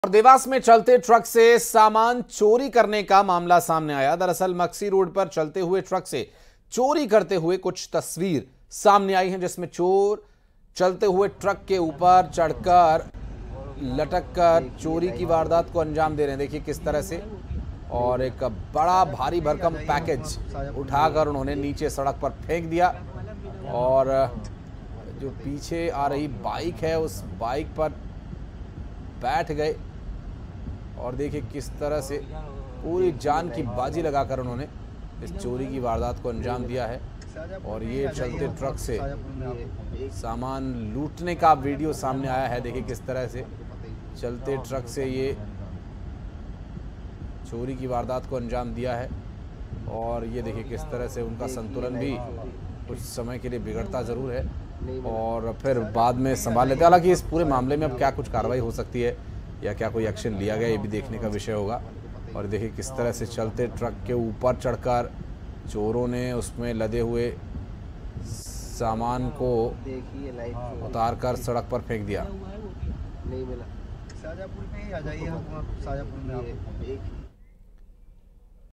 MP News : Dewas में चलते ट्रक से चोरी करने का आया मामला, वीडियो होश उड़ा देगी! | Breaking News
खबर है कि Dewas में चलते ट्रक से चोरी करने का आया मामला, वीडियो होश उड़ा देगी!
जहां पर चलते ट्रक से कुछ अज्ञात चोर चोरी कर रहे हैं